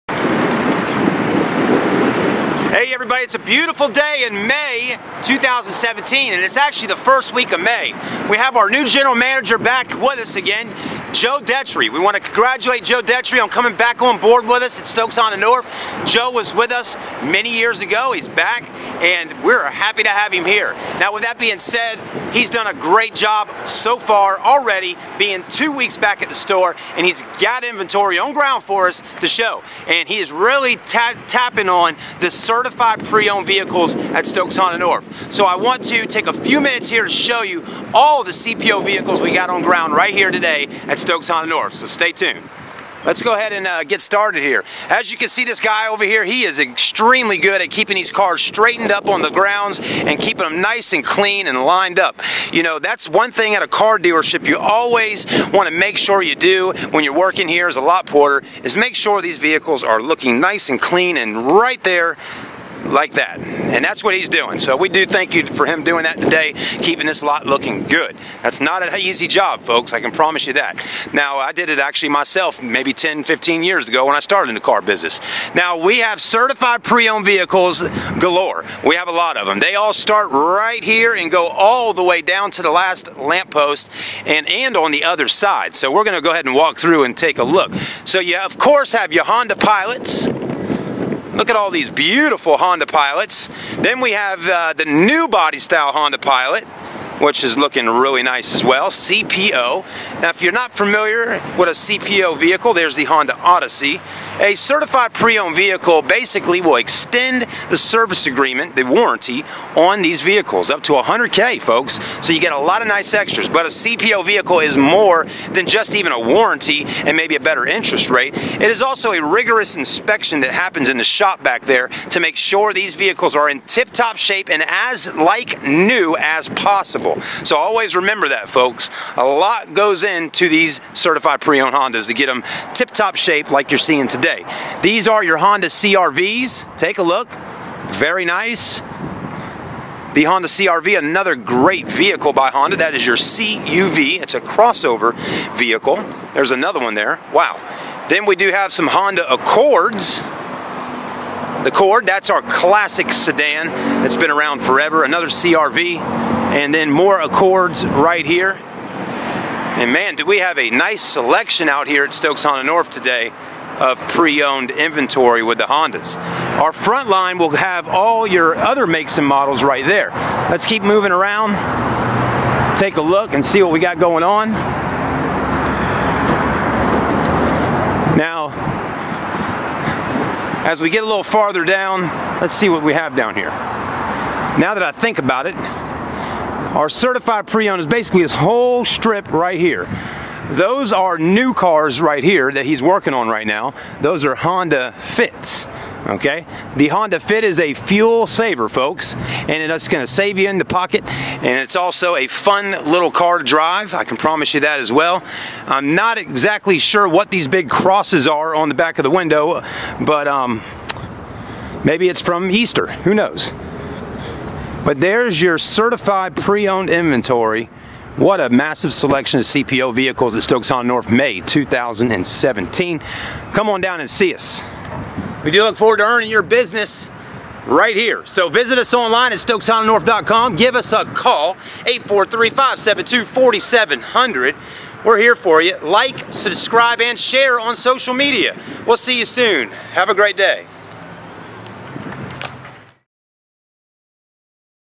New Shipment of Certified Preowned Honda's @ Stokes Honda North - May 2017
Hey Everyone. If you live near Charleston, SC and looking for a used Honda check out Stokes Honda North. We have a great selection of CPO Honda's.

Uploading new videos daily. Over 2700 Reviews to watch!